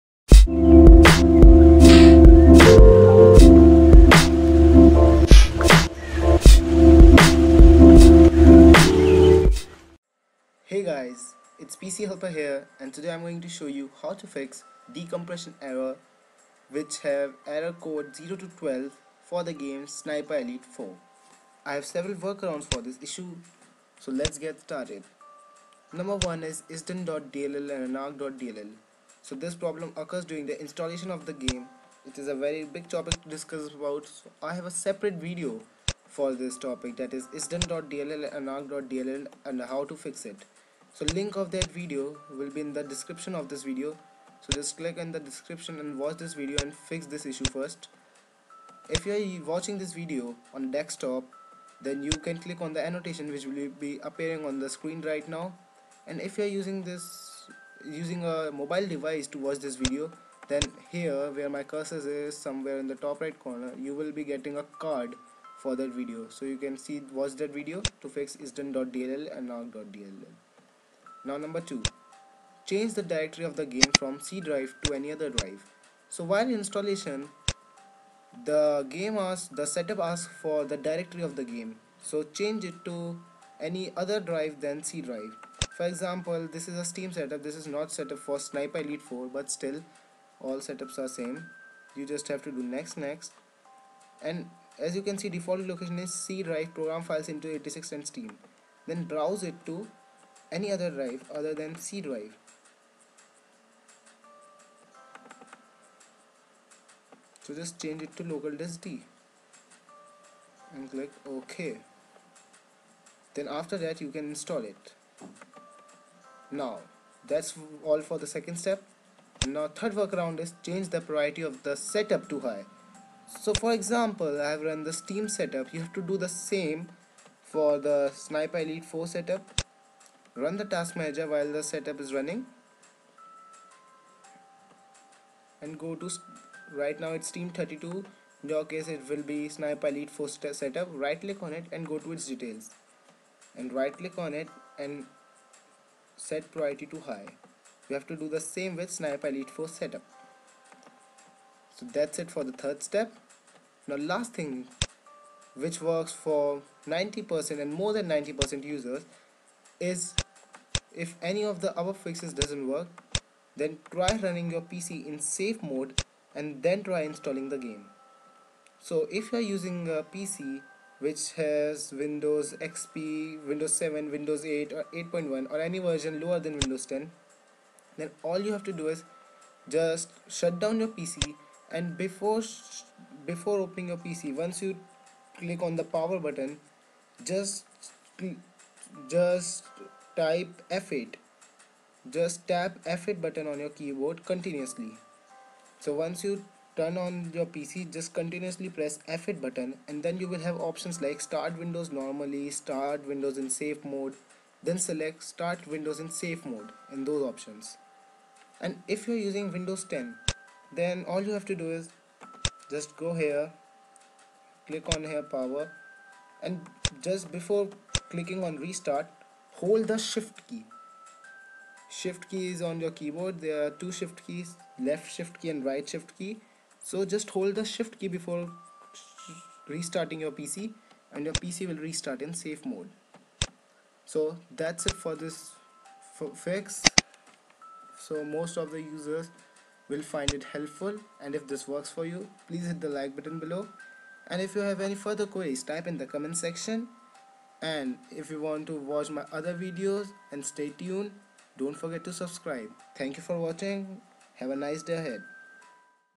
Sniper Elite 4 : Decompression Failed Error Code (0-12) [FIXED] [WORKING]
How to fix Decomression error for the game Sniper Elite 4.